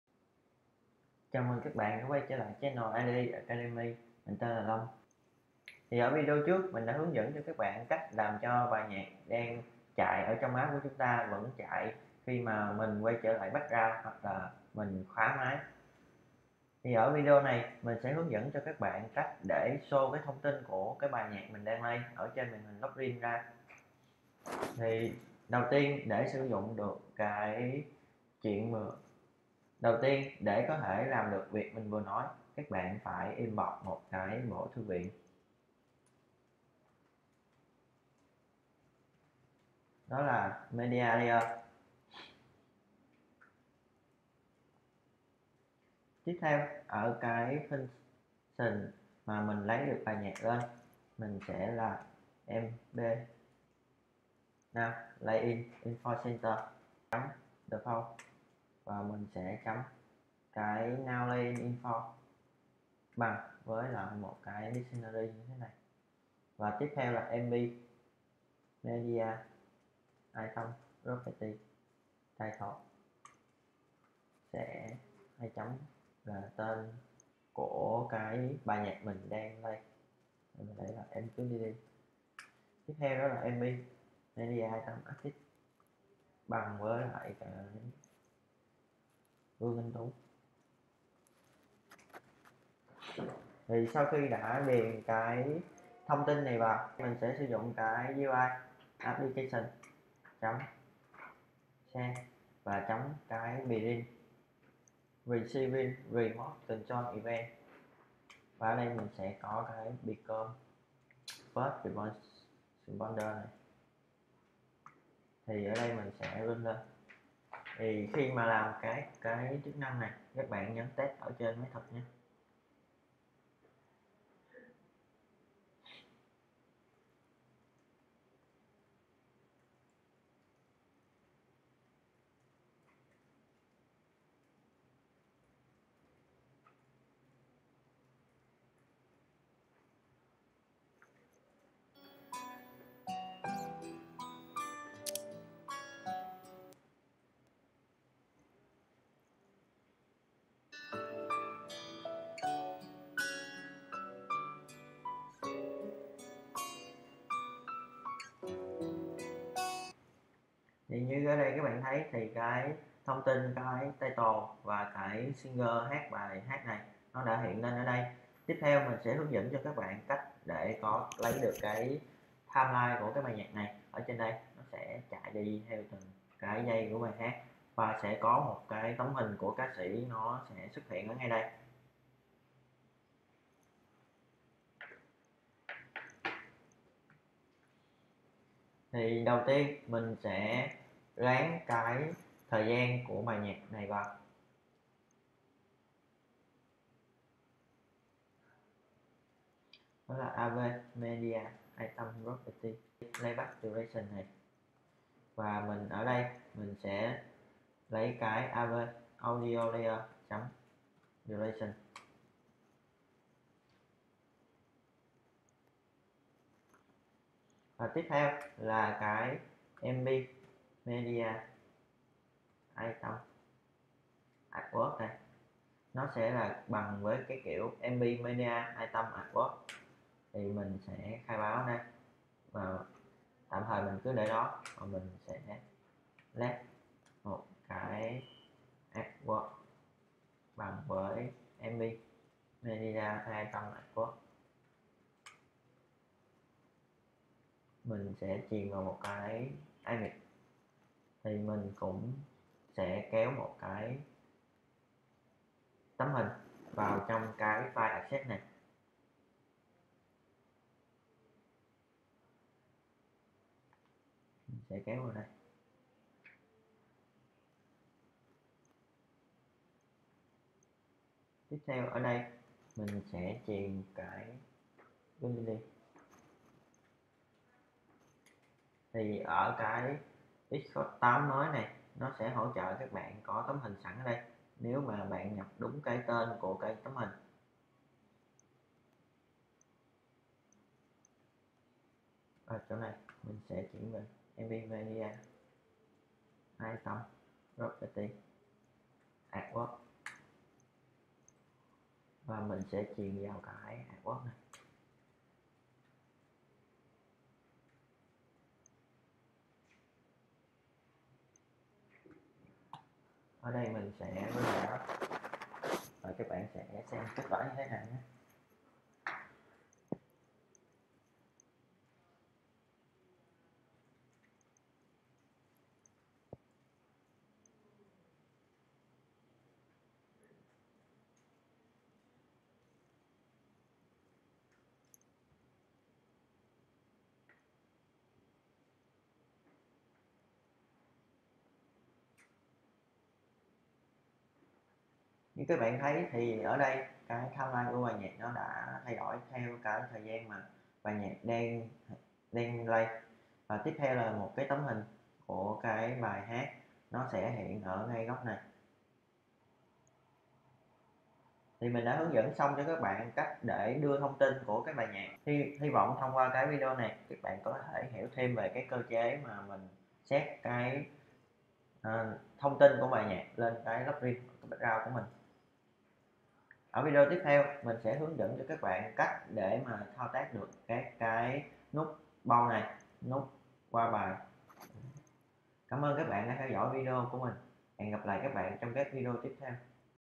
[AppMusic] 5. Info Music in Lockscreen
Bộ clips kỹ thuật làm app nghe nhạc với Swift 3 do IDEA biên soạn hướng dẫn các bạn từng bước làm được app nghe nhac cho riêng mình :).